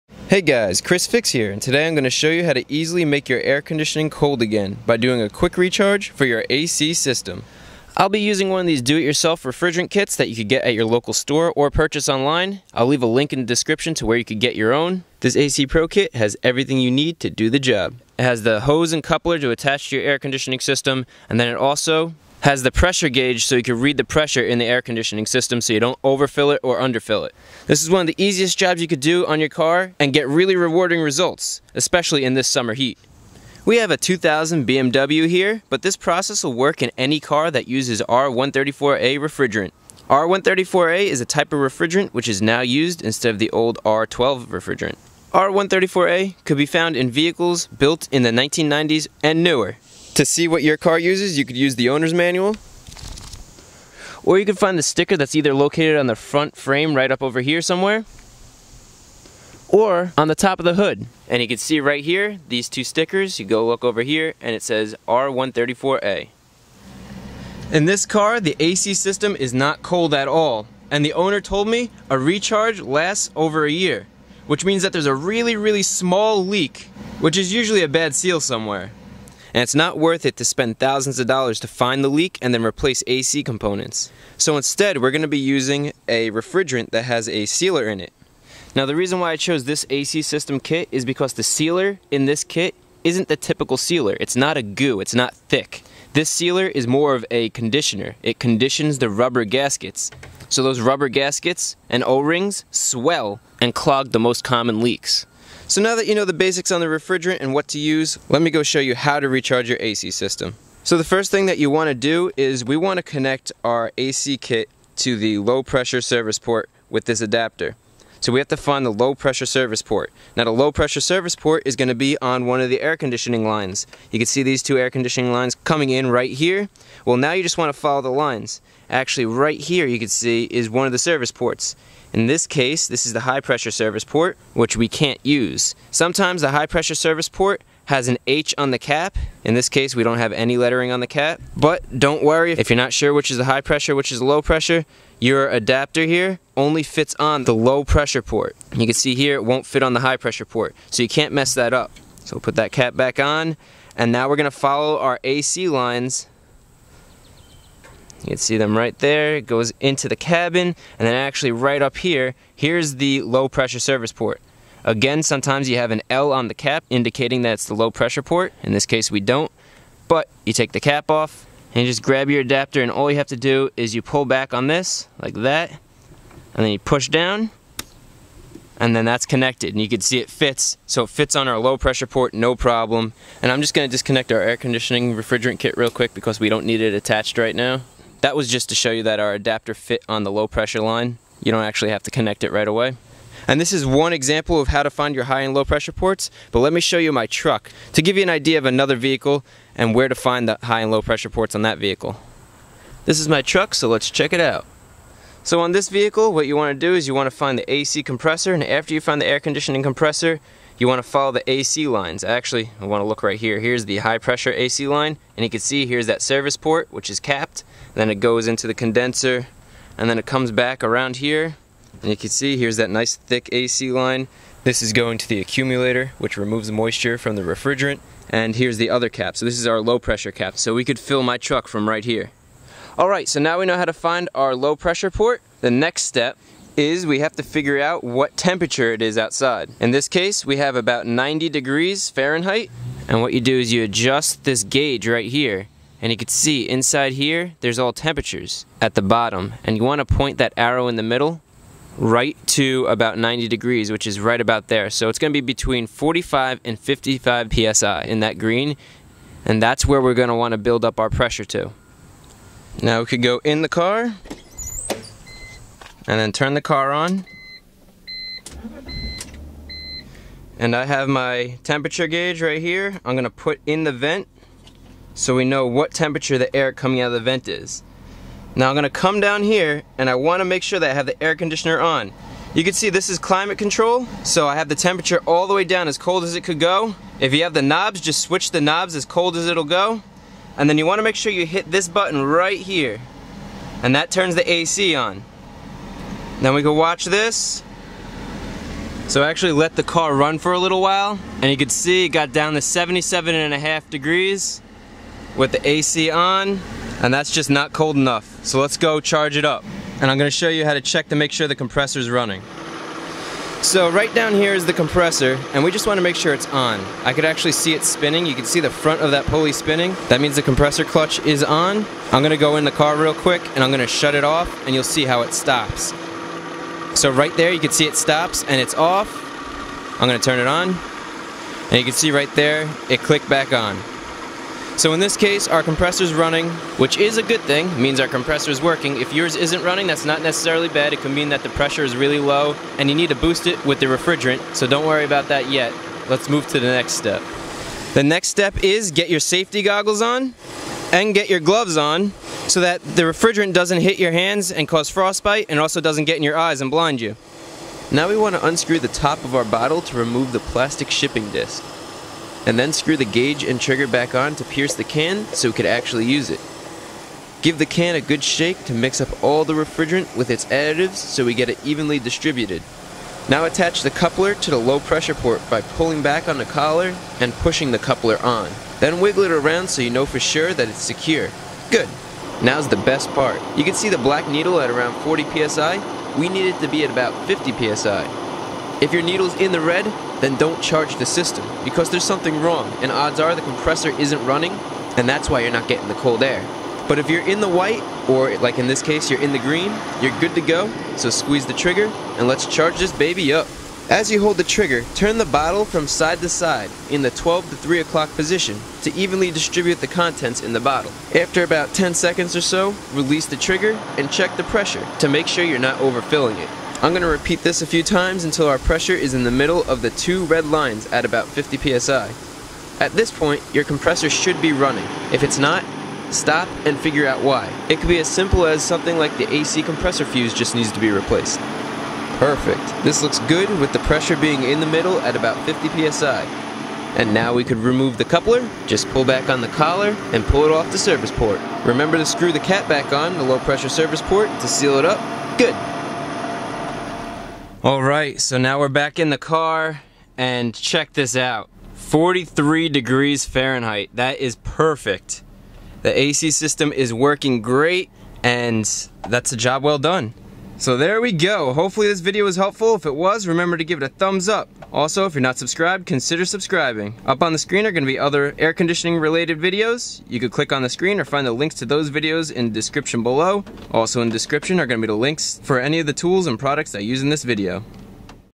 How to Recharge Your Car's AC System (Fast and Easy)
How to recharge AC in a car. Want cold air in your car this summer? I show you a quick and cheap way to get cold air if you have a slow leak in your AC system by recharging your AC system with refrigerant. I chose this refrigerant because the sealer in this product is a conditioner so it wont damage the ac system or the ac recovery machines mechanics use.

Thermometer I used